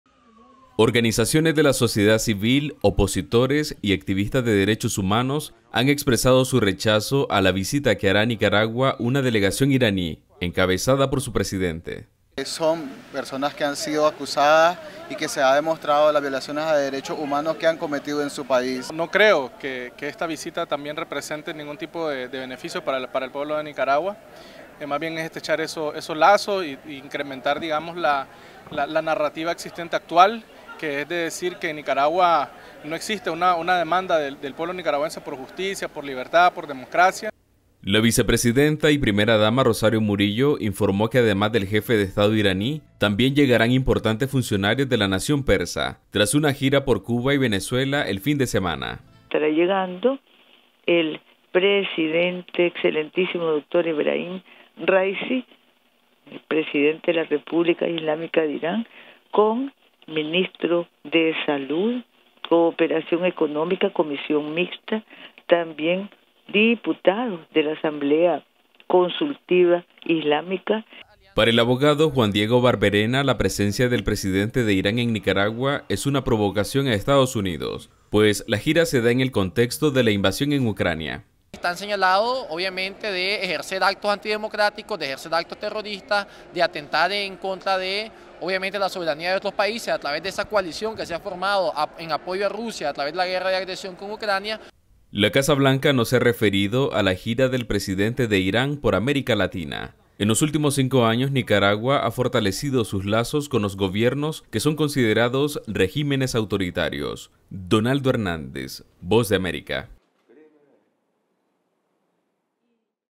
Presidente de Irán Ebrahim Raisi visitará Nicaragua
El presidente de Irán, Ebrahim Raisi, arribará a Nicaragua el próximo martes, según informó Managua. El dignatario persa se reunirá con su homólogo Daniel Ortega, un encuentro que algunos analistas ven como una provocación a Estados Unidos